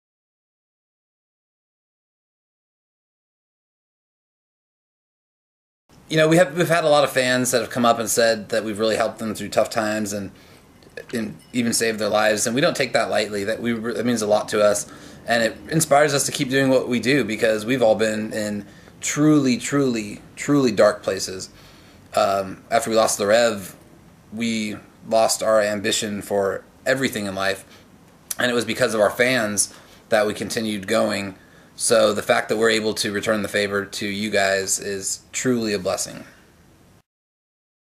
A7X Italia intervista gli Avenged Sevenfold - pt 8
(Second version uploaded with copyright)
Do not steal & reupload it. The other videos will be reported to You Tube.

L'A7X Italia ha avuto la possibilità di fare 8 domande, selezionate fra quelle dei fans italiani, alla band. Eccone le risposte.

Ottava domanda:
Cosa rispondete ai fans che vi dicono che la vostra musica ha salvato la loro vita?

Cosa rispondete ai fans che dicono che avete salvato loro la vita?
ZV: abbiamo molti fans che hanno detto che li abbiamo aiutati nei momenti duri, persino salvato la vita. Significa molto per noi, ci ispira a continuare con il nostro lavoro perchè anche noi abbiamo avuto un periodo molto molto buio. Dopo la perdita di Jimmy avevamo perso molte ambizioni della nostra vita. E grazie ai nostri fans andiamo avanti. Il fatto che siamo in grado di render loro il favore è una vera benedizione...